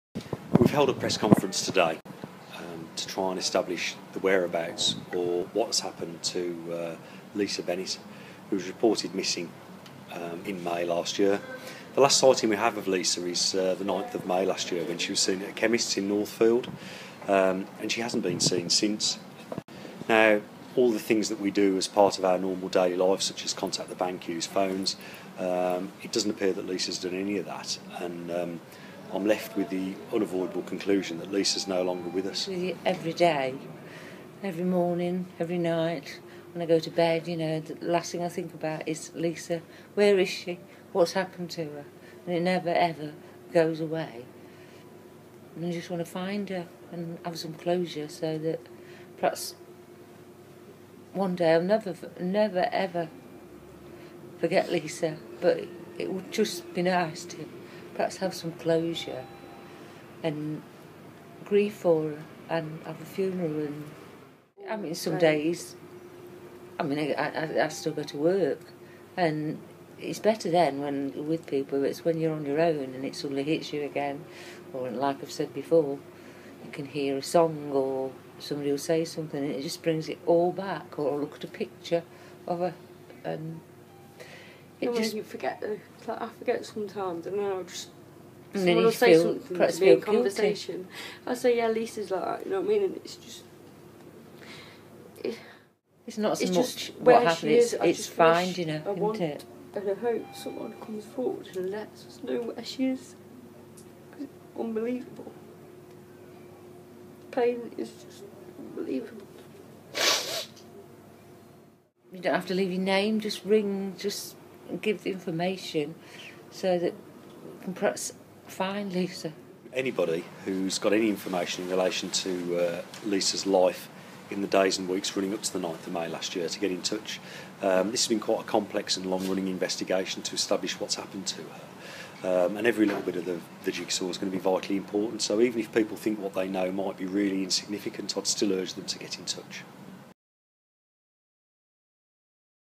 Detectives investigating the disappearance of Lisa Bennett are appealing for information.
Lisa Bennett, aged 39, was last seen in a chemist in Northfield on 9th May 2013.

Officers have carried out extensive enquiries from the time she went missing but failed to find any evidence that Lisa is still alive.

A man, aged 34, was arrested on suspicion of Lisa's murder yesterday morning (05/02). He has been released on bail pending further enquiries.

DI Warren Hines, who is lending the investigation, said: "Lisa had been living in the Selly Oak area before she went missing. She was a well loved Daughter and sister. Lisa was a complex character. Our big fear is that she was taken advantage of. We need anybody with information to come forward."

Lisa is described as being white, around 5ft 8ins tall with short dark brown hair and has a tattoo of a flower on the back of her neck and stomach.

Lisa's mother Janet Bennett, said: "All Lisa's family and friends who know her in Burton love her to bits. All we want to do is find out what has happened to her. Even though she can be a bit of a rebel, she is a lovely girl and we all miss her very much."

Please WMP on 101 if you have ANY information.